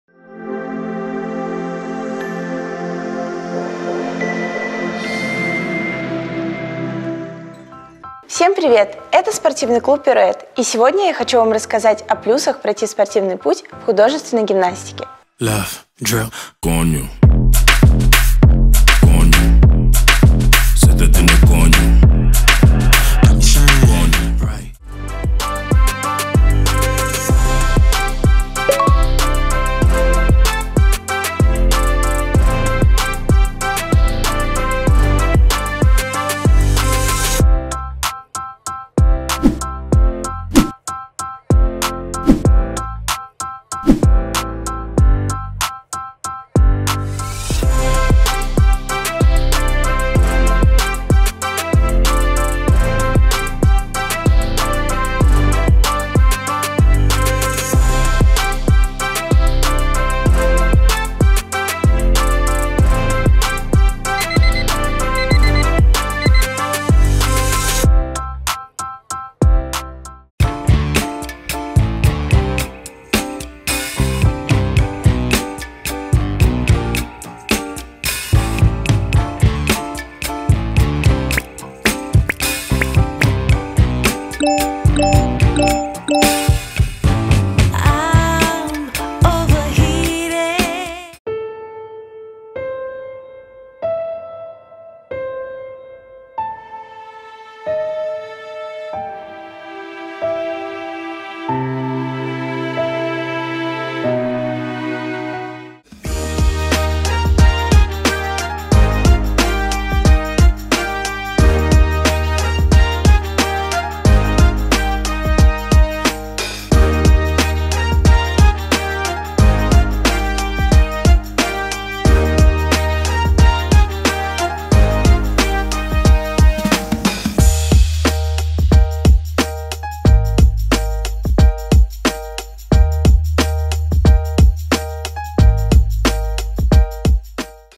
КАКИЕ ПЛЮСЫ пройти спортивную карьеру в художественной гимнастике? PIROUETTE
Спортивный клуб художественной гимнастики PIROUETTE